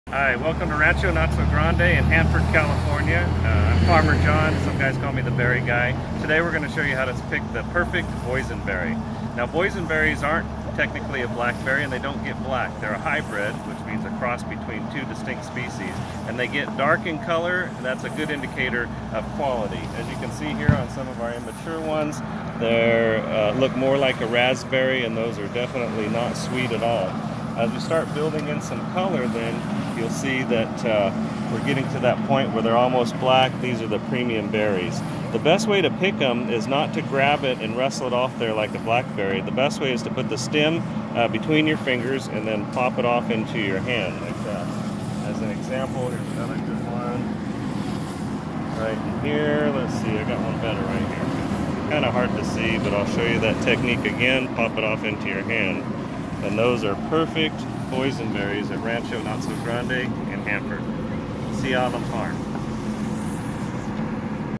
How to pick the perfect Boysen berry
On the farm at Rancho Notso Grande